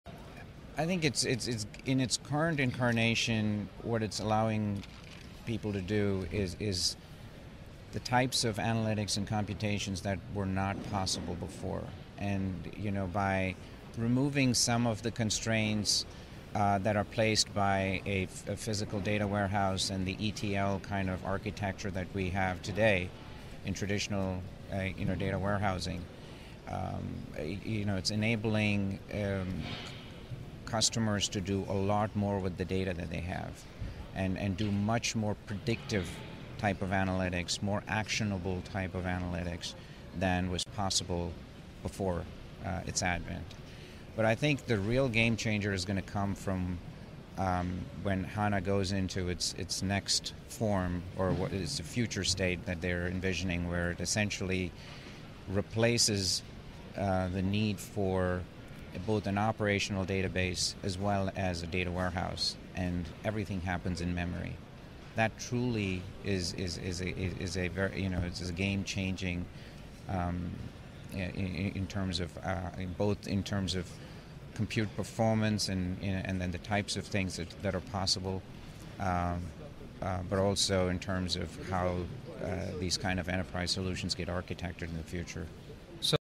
Dell's Sukumar Narayanan Describes HANA's "Fast Data" Approach
Dell's Sukumar Narayana describes the benefits of HANA, SAP's new in-memory analytics appliance.